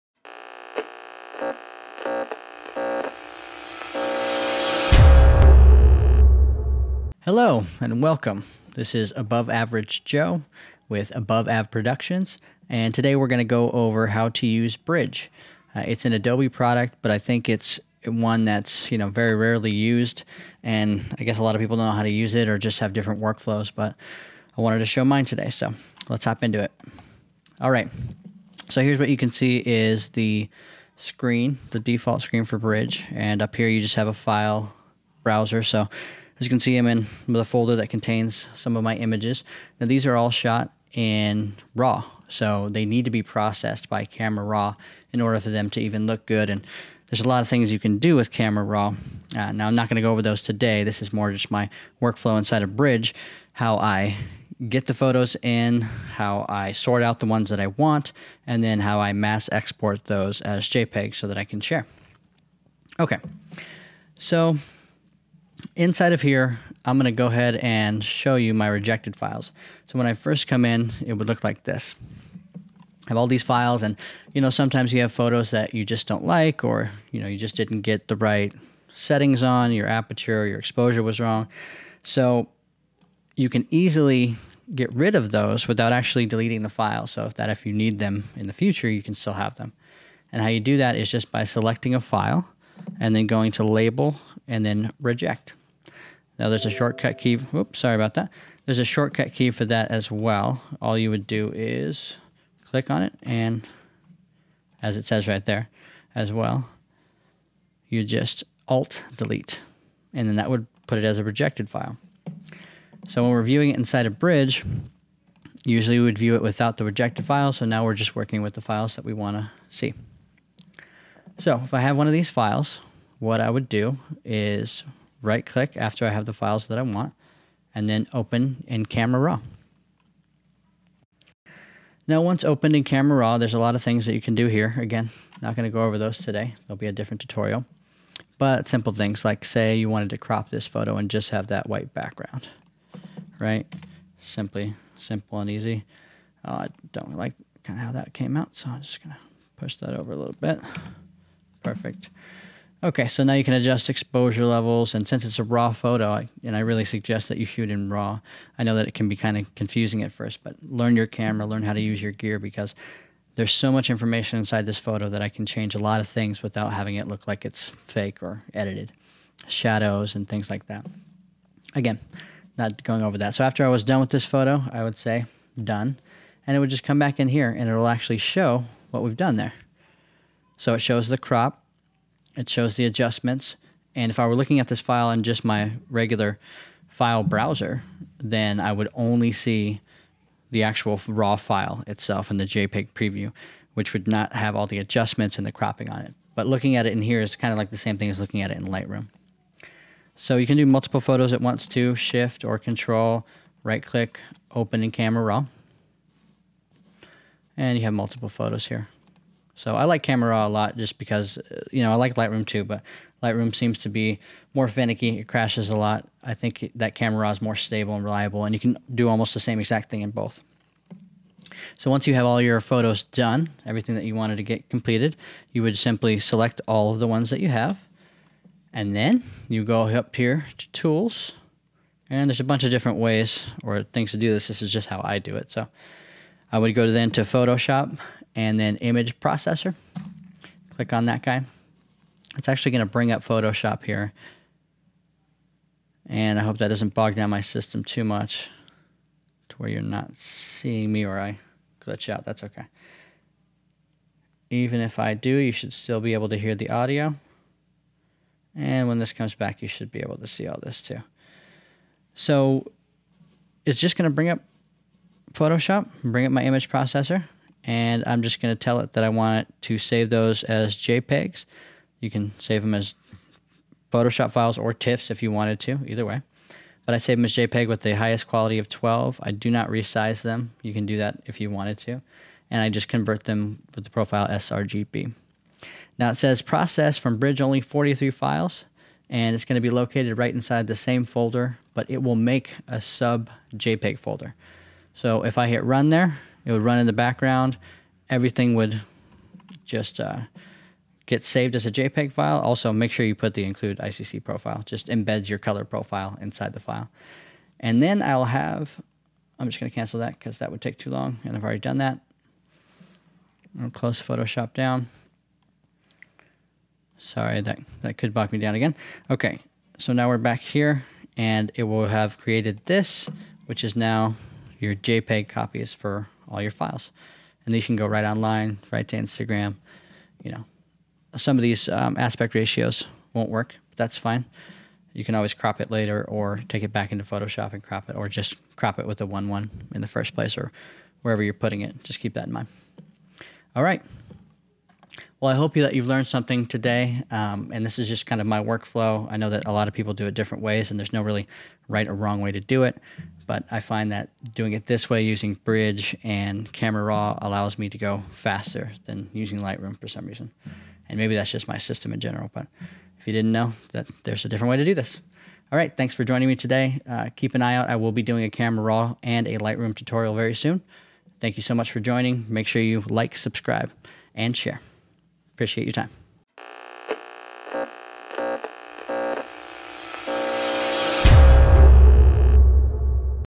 Adobe Bridge and Camera Raw workflow
I wanted to show you all my photo editing workflow. Bridge and Camera Raw from Adobe are powerful tools. This is how you can use them easily and efficiently.